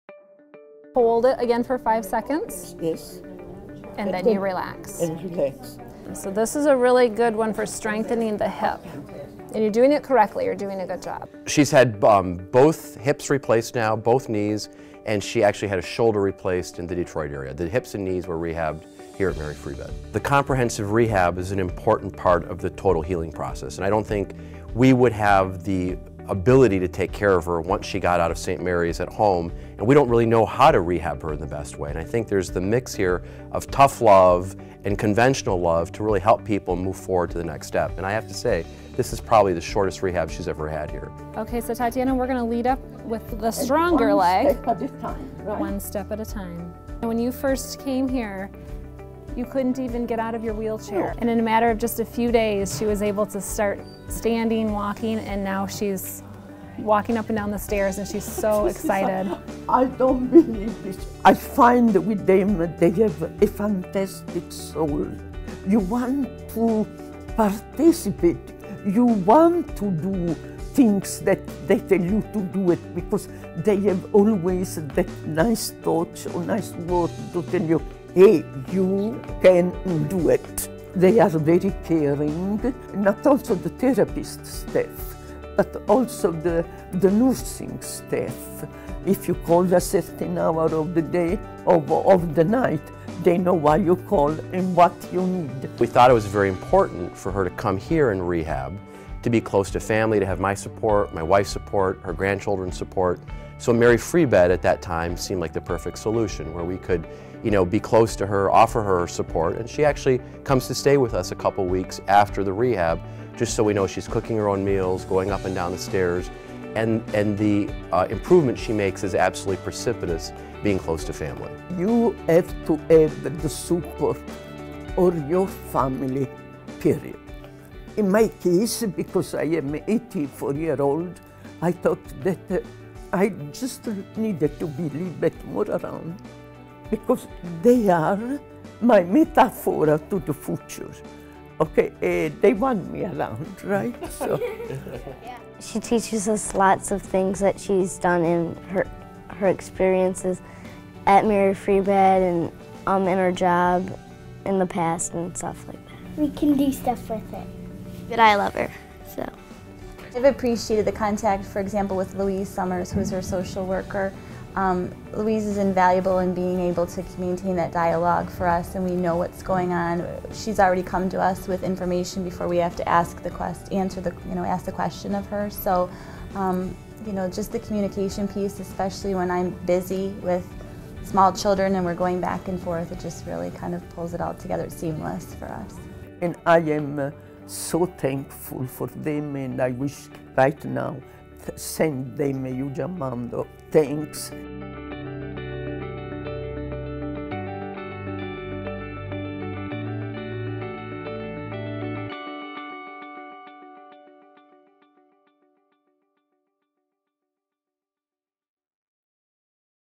Patient Supported by Family, Therapists and Nurses at Mary Free Bed
Tatiana Padula is the traditional Italian grandmother who graciously gives warm hugs and smiles to anyone that greets her, and who symbolizes family. When she was diagnosed with osteoarthritis she wanted to relieve her pain quickly so she could enjoy her time with her grandchildren. Joint replacement surgery was Tatiana's best option. For recovery, her doctor and son, who is also a doctor, recommended Mary Free Bed Rehabilitation Hospital. Her experience at Mary Free Bed went so well the first time, Tatiana came back three more times over the course of ten years to recover from her hip and knee replacements. "Mary Free Bed was part of my team during recovery and is now part of my family," shares Tatiana.